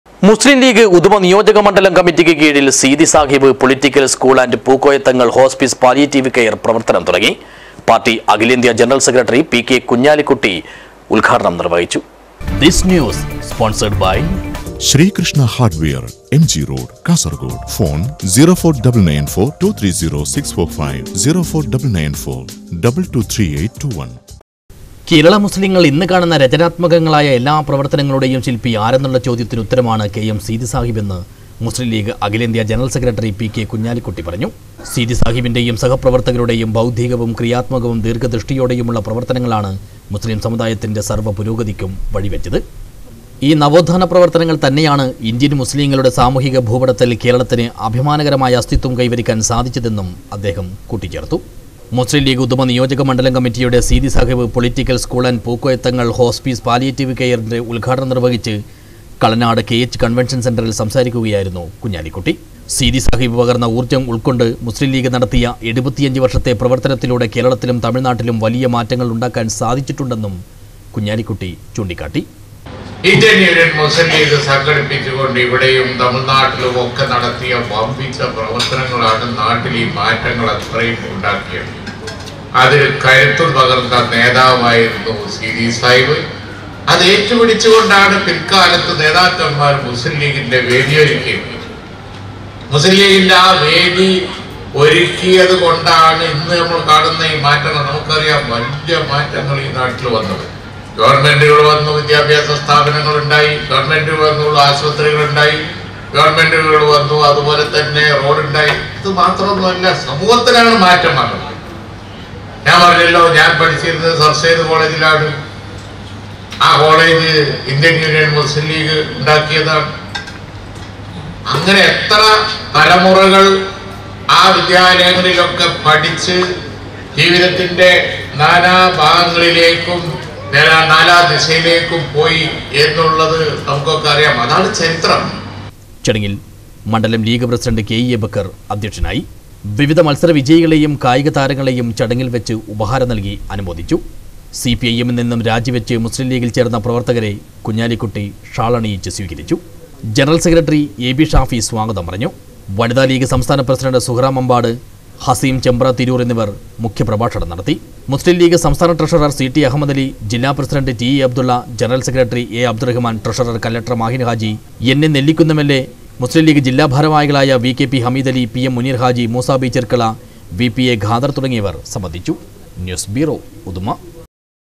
മുസ്ലീം ലീഗ് ഉദുമ നിയോജക മണ്ഡലം കമ്മിറ്റിക്ക് കീഴില്‍ സീതി സാഹിബ് പൊളിറ്റിക്കല്‍ സ്‌കൂള്‍.
മുസ്ലീം ലീഗ് ഉദുമ നിയോജക മണ്ഡലം കമ്മിറ്റിക്ക് കീഴില്‍ സീതി സാഹിബ് പൊളിറ്റിക്കല്‍ സ്‌കൂള്‍ & പൂക്കോയ തങ്ങള്‍ ഹോസ്പീസ് പാലിയേറ്റീവ് കെയര്‍ പ്രവര്‍ത്തനം തുടങ്ങി. പാര്‍ട്ടി അഖിലേന്ത്യാ ജനറല്‍ സെക്രട്ടറി പി.കെ കുഞ്ഞാലിക്കുട്ടി ഉദ്ഘാടനം നിര്‍വ്വഹിച്ചു.